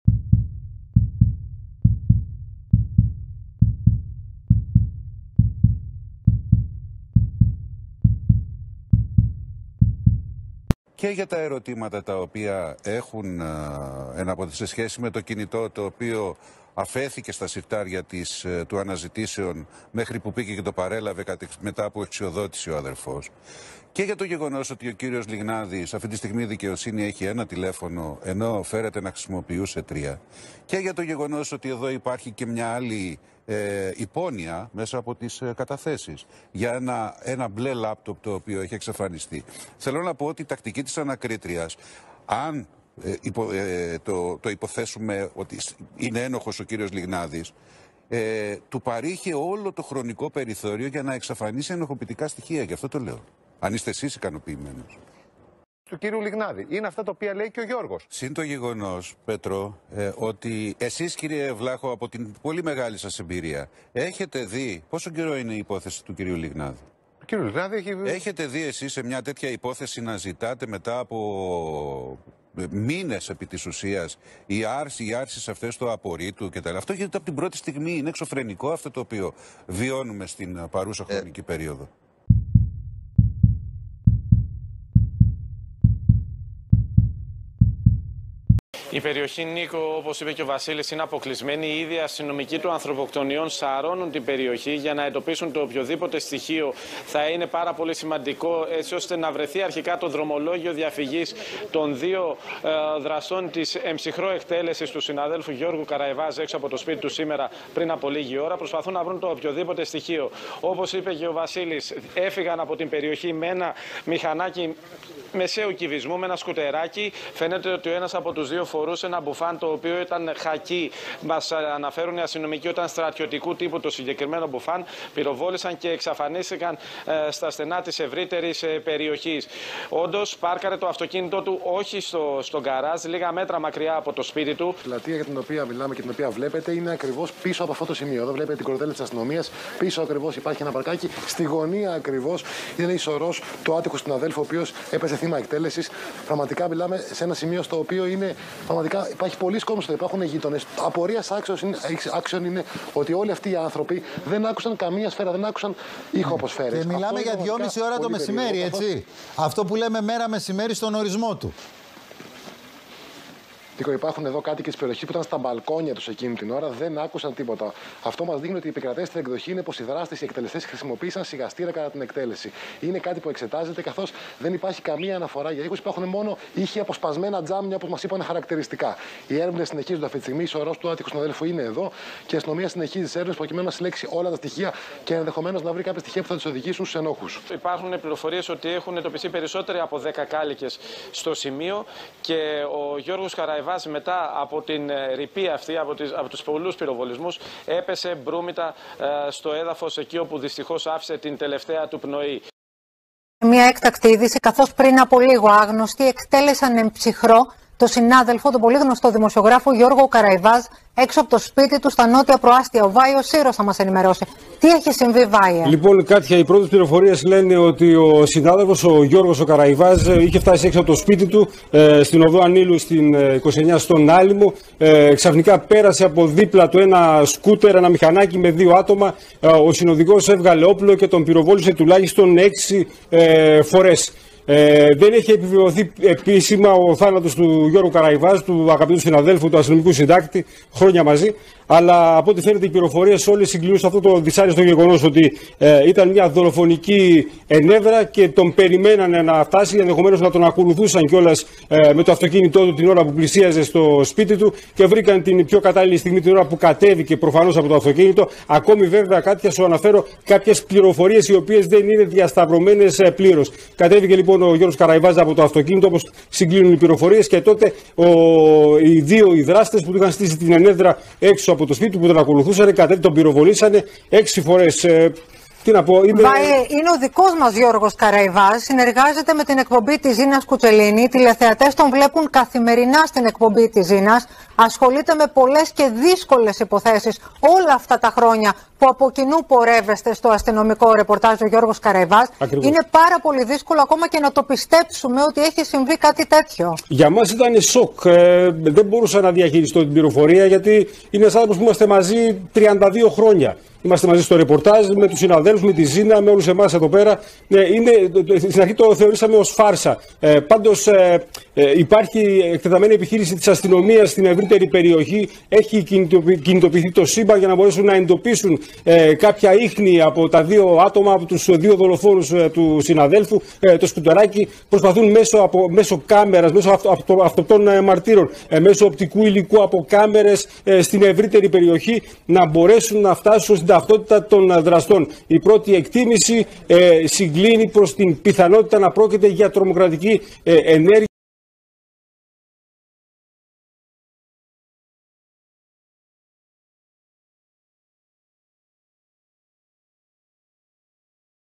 Εκτέλεσαν τον δημοσιογράφο Γιώργο Καραϊβάζ uhutv
Συλλυπητήρια στην οικογένεια του και τους αγαπημένους του.